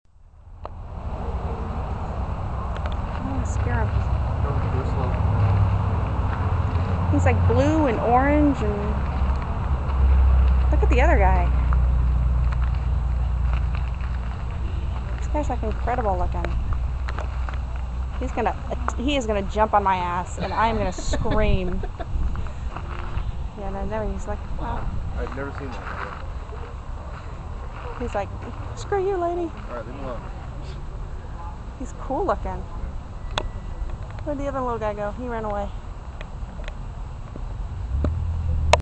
African Rainbow Lizard
We saw this guy at Coral Caste in S. Florida